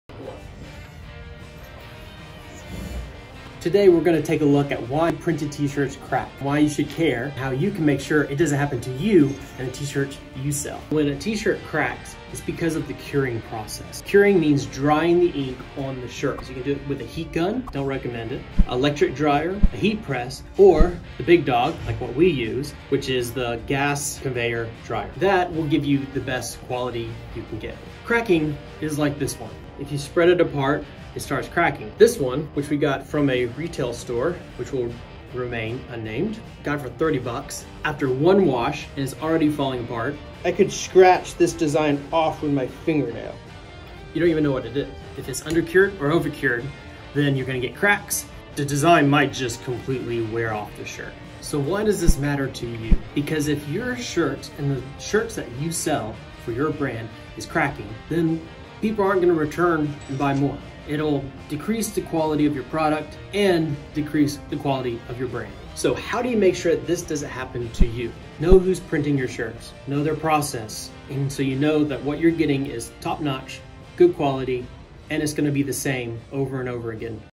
Why do your shirt designs CRACK? (And what to do about it)
Josh breaks down why t-shirt designs crack and a couple steps you can take to prevent it from happening!